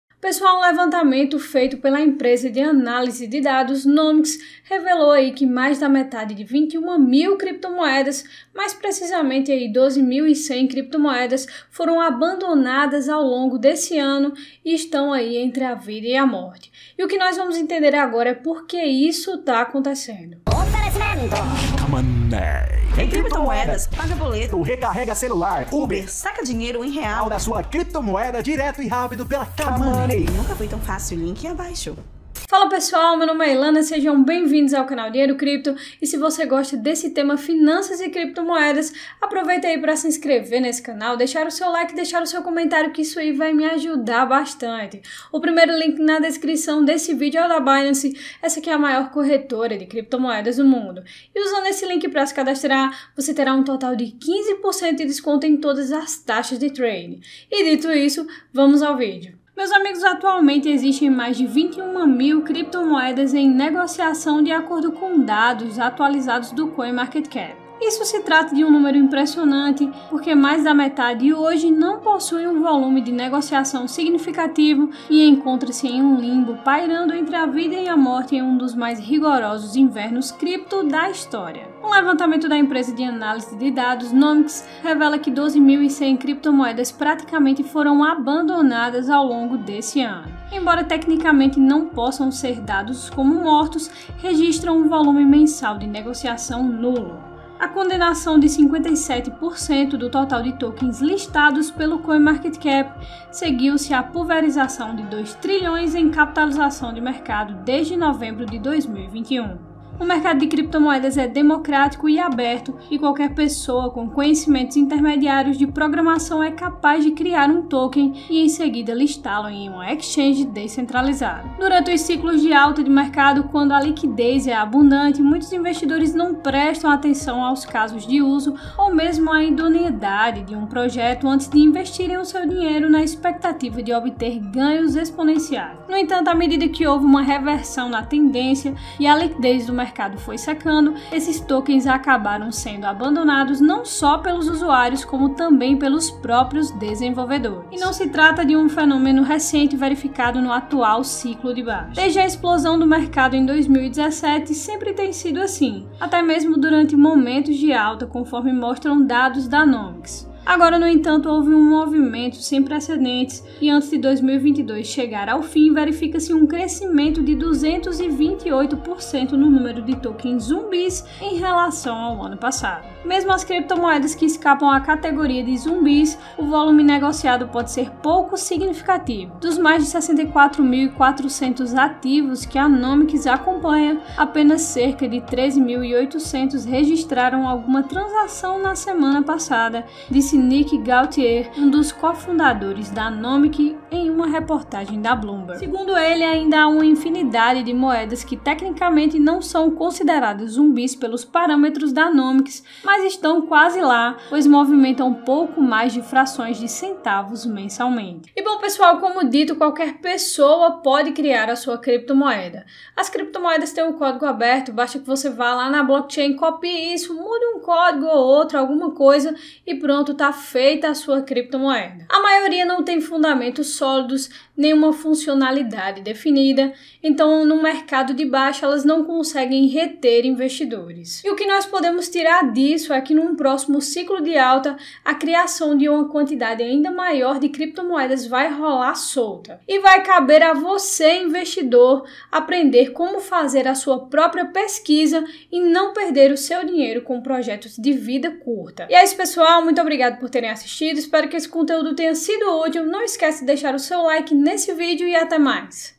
INVERNO CRIPTO CONDENA 12.100 CRIPTOMOEDAS AO LIMBO DOS 'TOKENS ZUMBIS'!
Um levantamento da empresa de análise de dados Nomics revelou que mais da metade 21 mil criptomoedas foram abandonadas ao longo desse ano estão entre a vida e a morte!